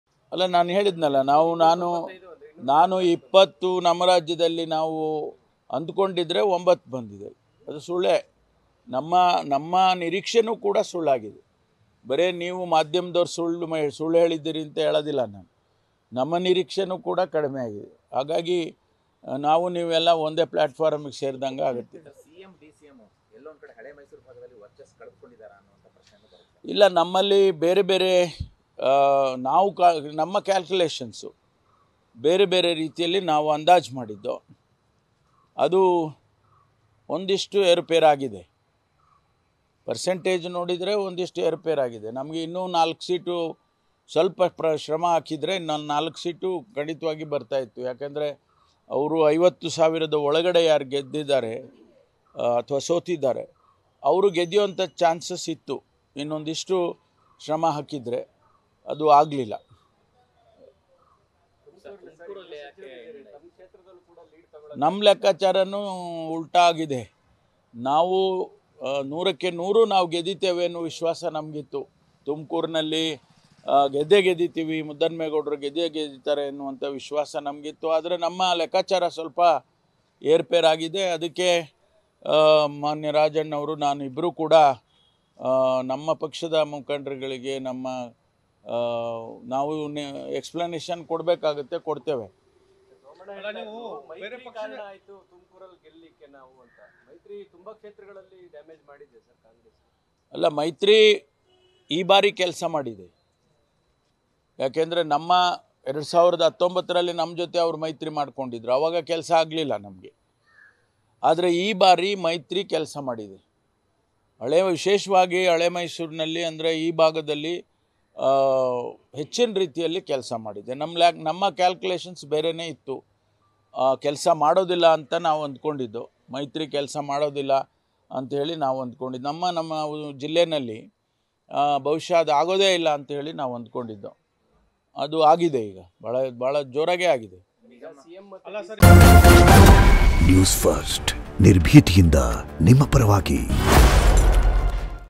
G Parameshwara : ಇನ್ನು ಸ್ವಲ್ಪ ಎಫರ್ಟ್​ ಹಾಕಿದ್ರೆ ನಮ್ಗೆ ಇನ್ನೂ 4-5 ಸೀಟ್ ಬರ್ತಿತ್ತು | Congress
G Parameshwara : ಇನ್ನು ಸ್ವಲ್ಪ ಎಫರ್ಟ್​ ಹಾಕಿದ್ರೆ ನಮ್ಗೆ ಇನ್ನೂ 4-5 ಸೀಟ್ ಬರ್ತಿತ್ತು | Congress | Lok Sabha Election 2024 Result | @newsfirstkannada